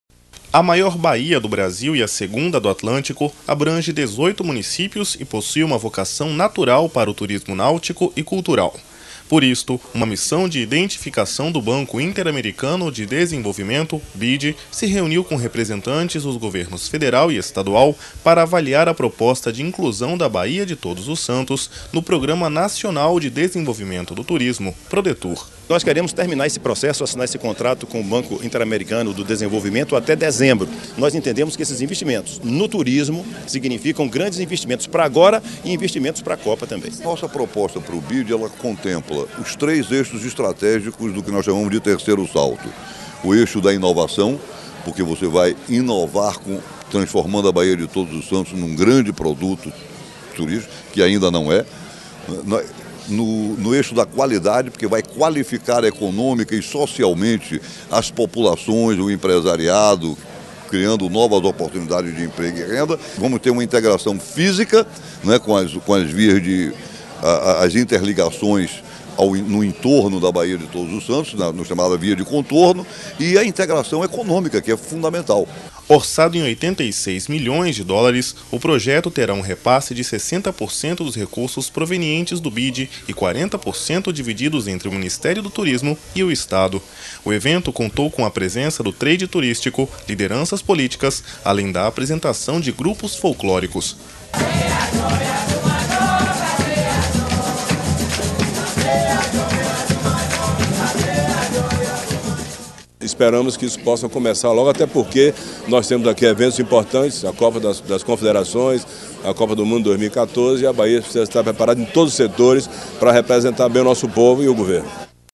Investimento do BID vai estimular turismo na Baía de Todos-os-Santos
A Bahia está adiantada no processo de captação de US$ 86 milhões para investimento na área de turismo da região da Baía de Todos-os-Santos (BTS), que serão disponibilizados com a inclusão do estado no Programa Nacional de Desenvolvimento do Turismo (Prodetur Nacional). Para isso, uma missão de identificação do Banco Interamericano de Desenvolvimento (BID) veio assistir, na manhã desta segunda-feira (16), no Centro de Convenções, a apresentação feita pela Secretaria Estadual do Turismo (Setur) sobre o plano de investimento que vai beneficiar mais de 3,4 milhões de habitantes em 18 municípios do entorno da BTS.